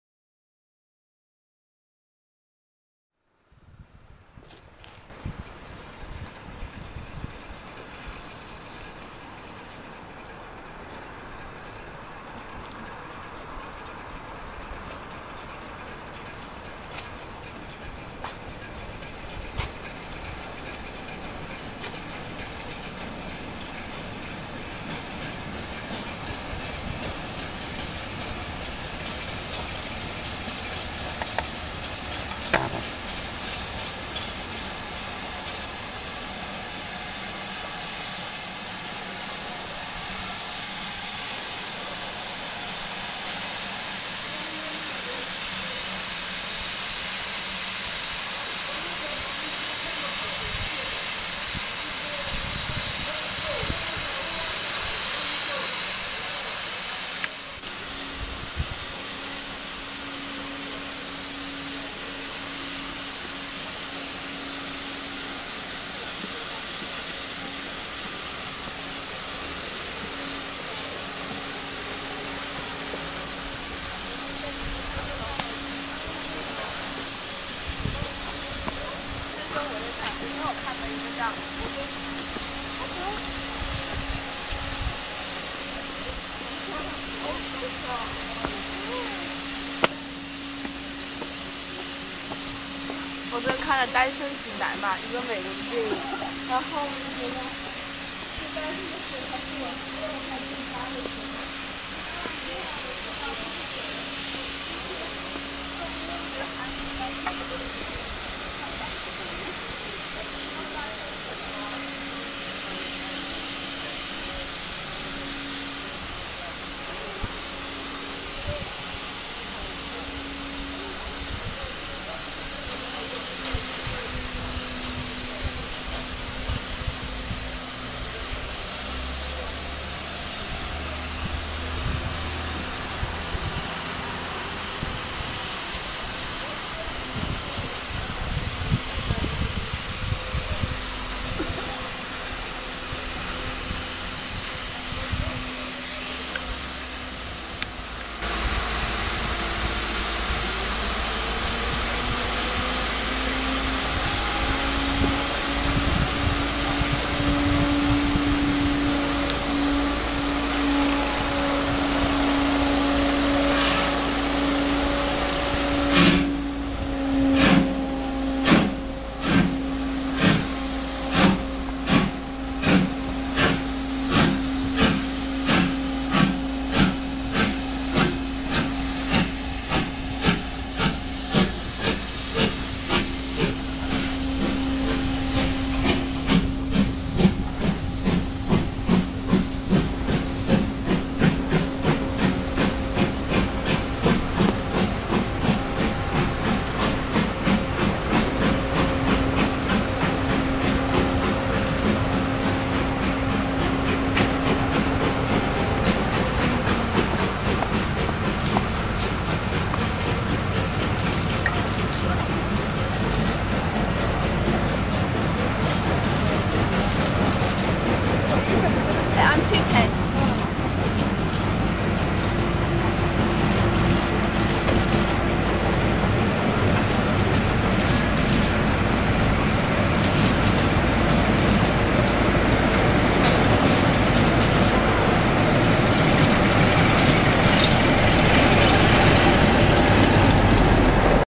Scarborough Spar Express
Scarborough Spar Express 25 October 2016 taking on water at Bootham